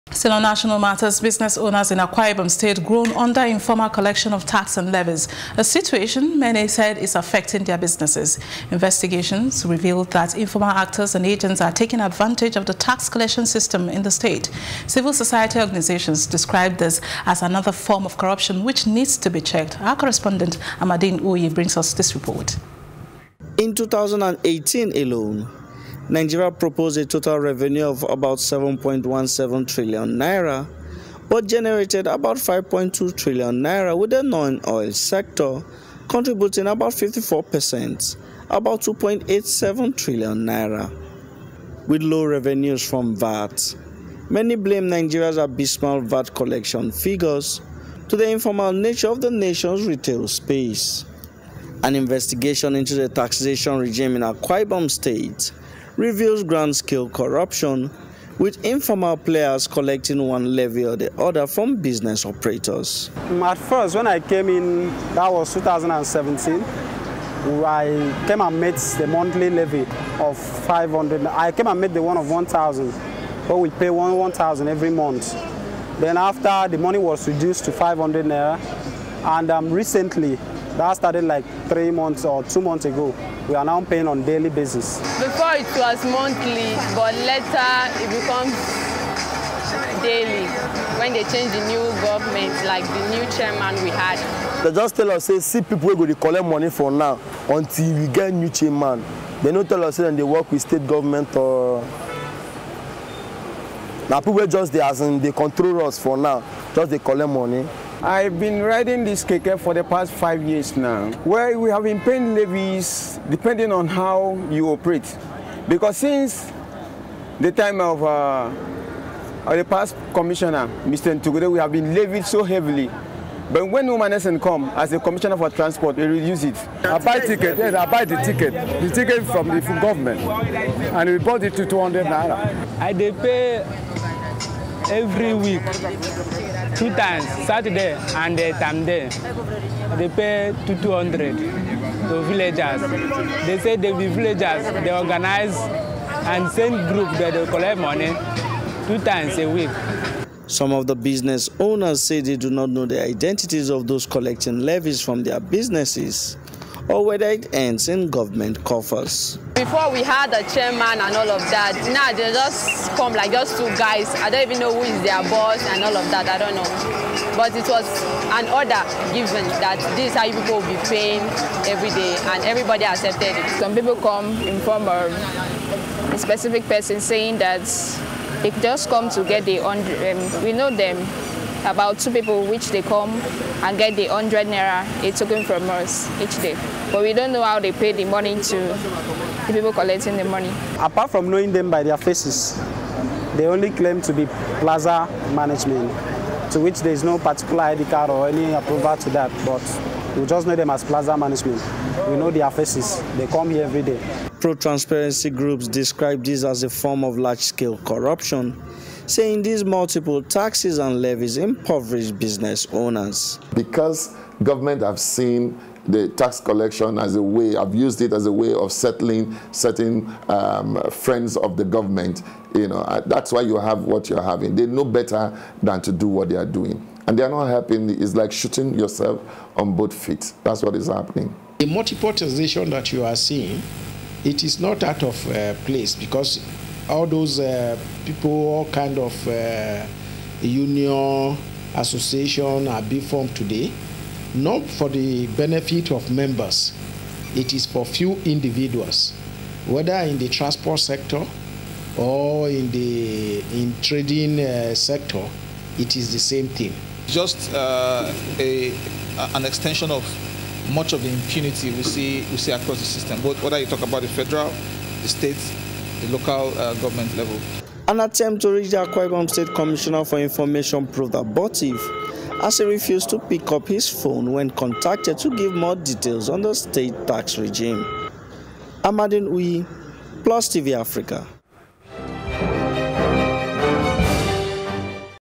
Illegal Tax Collections: Business Owners Lament In Akwa Ibom
Business owners in Akwa Ibom state groan under informal collection of tax and levies, a situation many said is affecting their businesses.

Investigation revealed that informal actors and agents are taking advantage of the tax collection system in the state. Civil society organizations described this as another form of corruption which needs to be checked.

Our correspondent Amadin Uyi brings us this report.